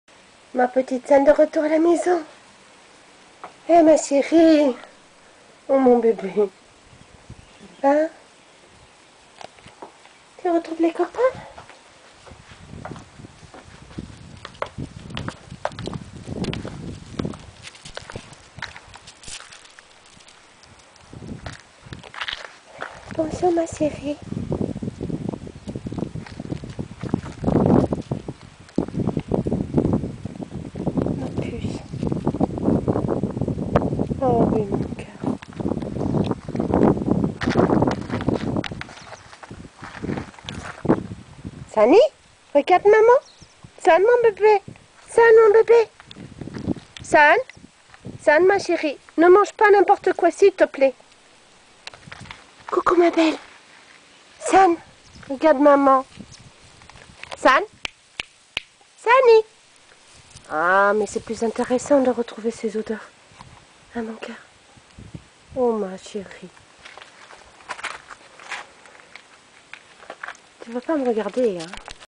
De retour à la maison
Quel bonheur de retrouver mon bébé après 24 heures d'angoisse à la clinique.
Je t'aime ma petite Sun, mon amour.